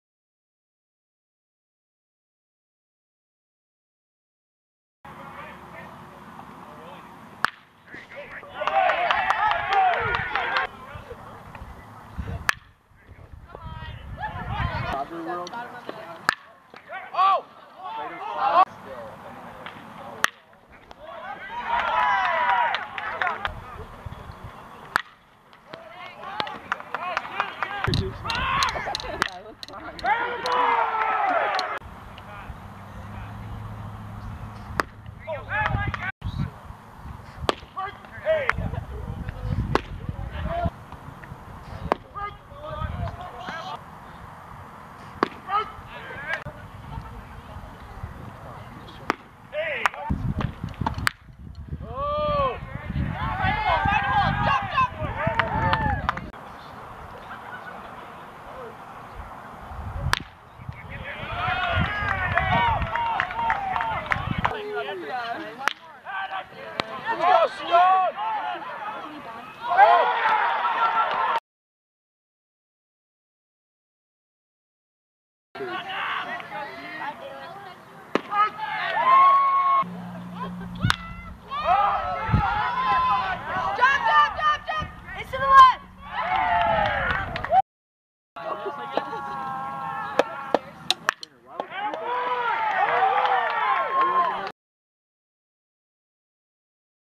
Pierre H.S. baseball @ Rapid City Central
Pierre H.S. baseball @ Rapid City Central on April 30, 2013.
(Highlights are from game 1 only)
Score: 14-11 Pierre